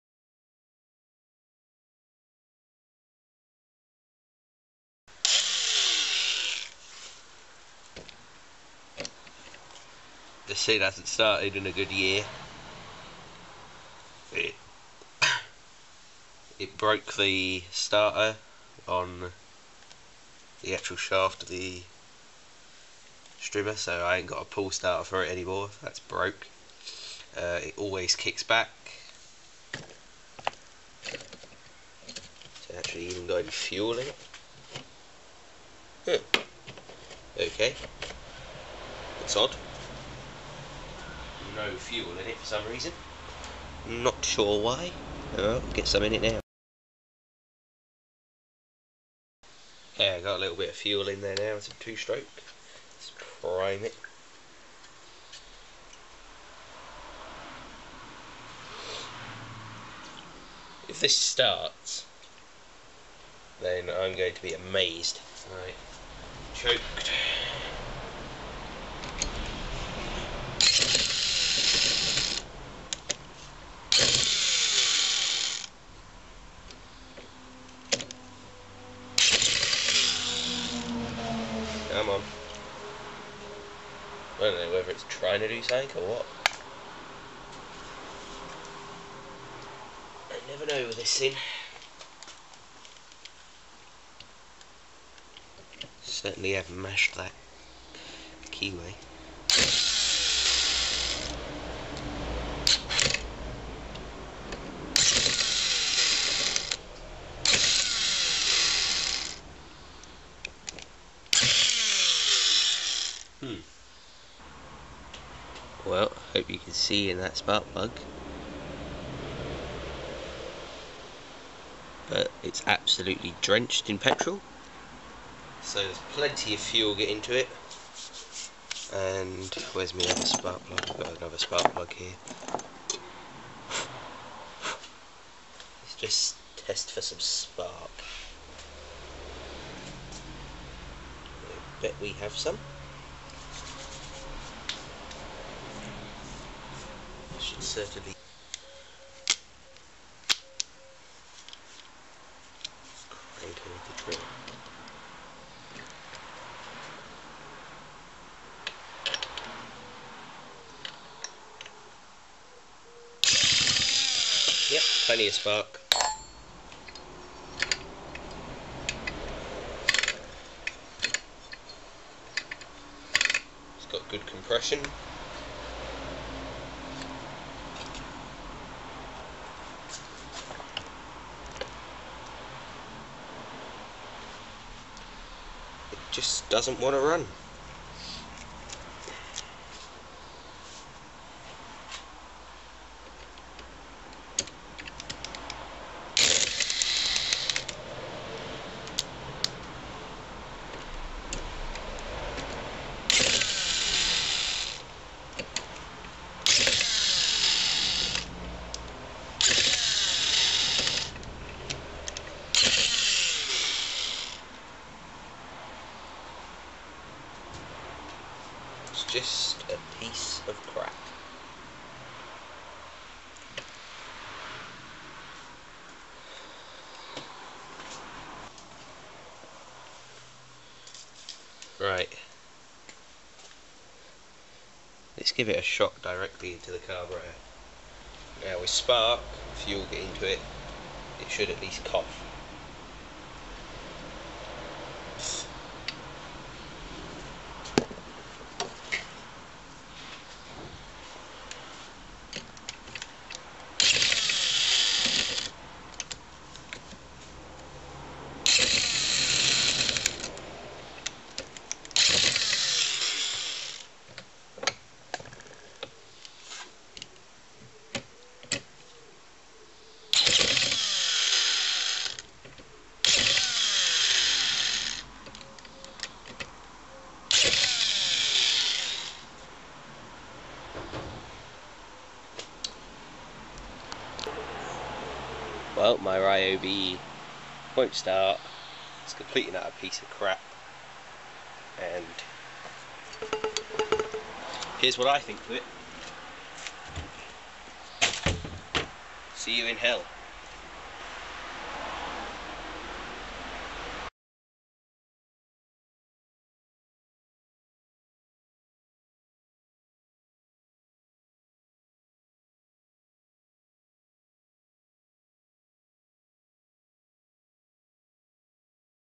Me Vs The Ryobi
This is the first and possibly last episode in my new series 'Me Vs The Ryobi'. The viewers will really be the driving force on this one, as I really dont want to waste my time making something nobody wants to see. So if you want to see more of this then comment below, you may also suggest an ending for the next episode :) but please keep it a little simple :L nothing along the ideas of this 'Can you please shot it with a 50 cal' Because i dont have one of them :L